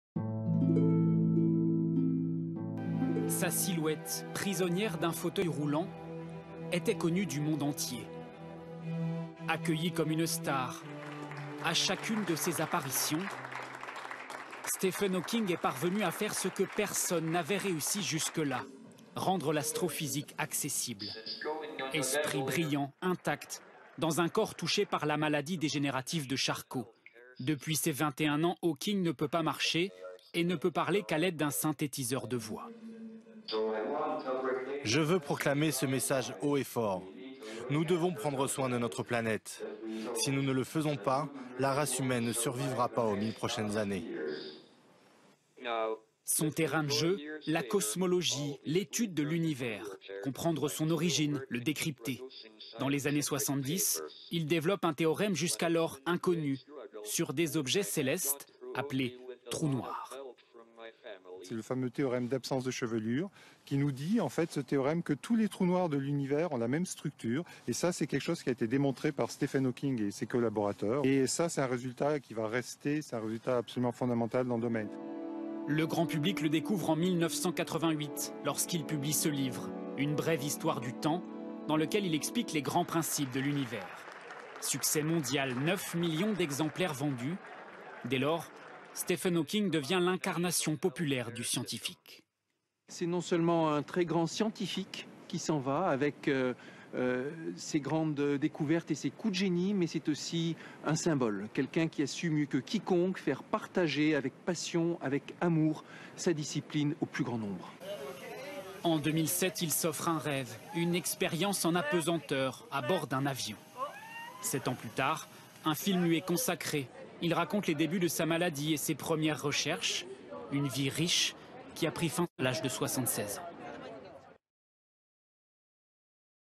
L'astrophysicien britannique Stephen Hawking a toujours été en avance sur son temps.
Stephen Hawking, réputé pour ses travaux sur les trous noirs, la cosmologie quantique et ses livres de vulgarisation sur les mêmes sujets, est mort le 14 mars à l'âge de 76 ans.

Connu du grand public à la fois pour son livre «Une brève histoire du temps» et pour la maladie de Charcot dont il souffrait, Stephen Hawking a également été professeur de mathématiques à l'Université de Cambridge de 1980 à 2009.

C'est le scientifique qui a réussi à faire aimer l'astrophysique au plus grand nombre. Facétie de l'histoire : Stephen Hawking s'est éteint le jour de la naissance d'Albert Einstein. Pourquoi restera-t-il une icône pour les stars de la high-tech comme les plus geeks d'entre nous ? Retour sur une vie de combats.